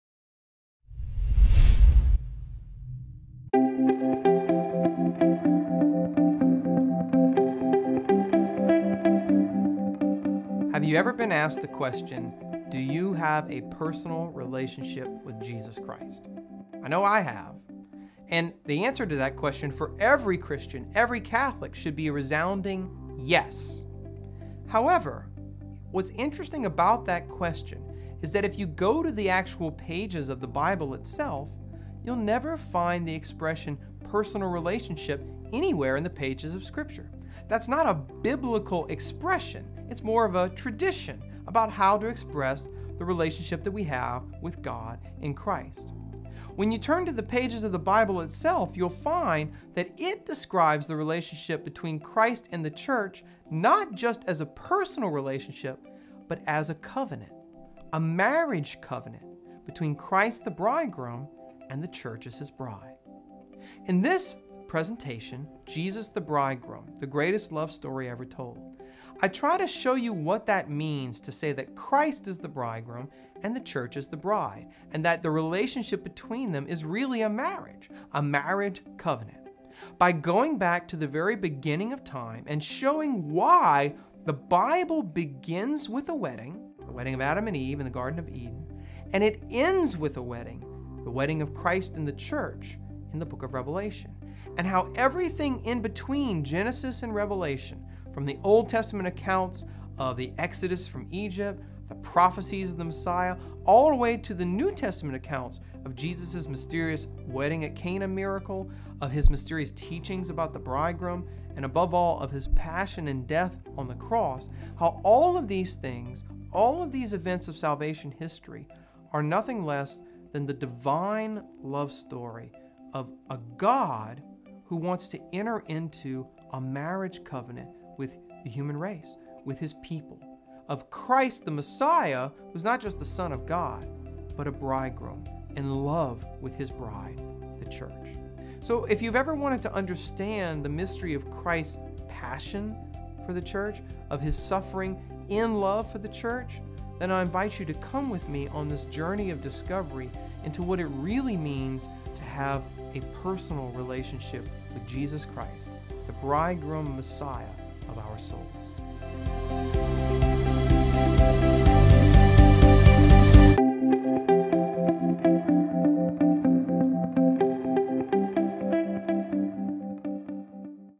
Jesus the Bridegroom [Single Presentation] (Intro)
Dr. Brant Pitre introduces his set on Jesus the Bridegroom.